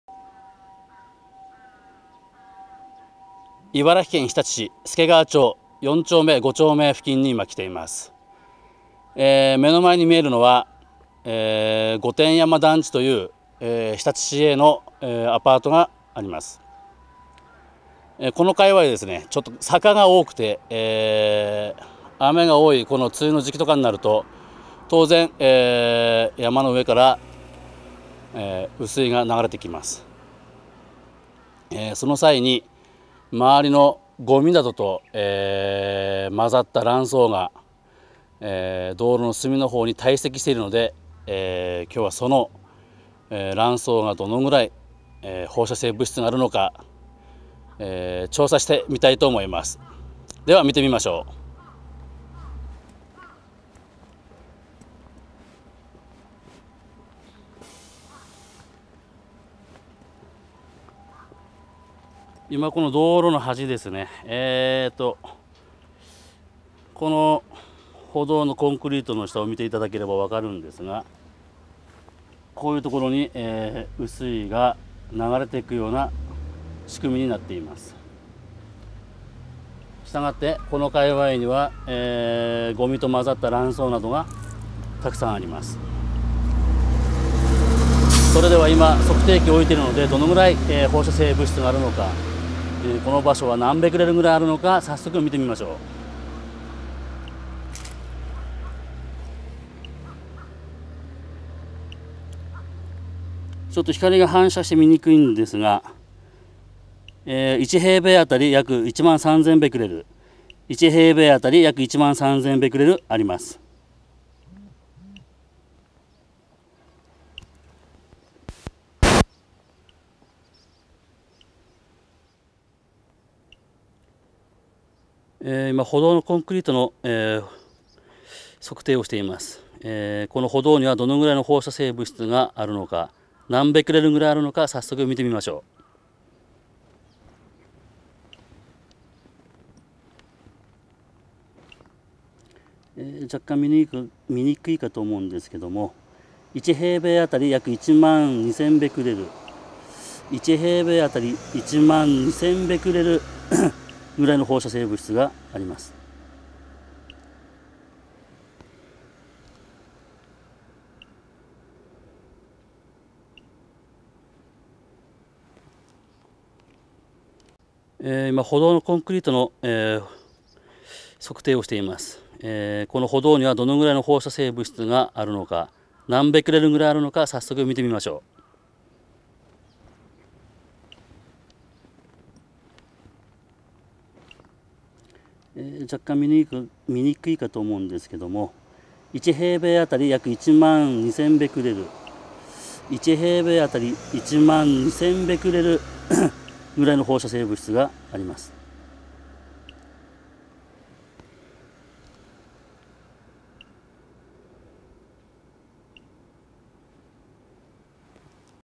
日常生活に潜む恐怖の放射性物質 茨城県日立市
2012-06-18

福島第一原発から約95キロメートル離れた日立市でこの事実を受け止める者たちは少ない。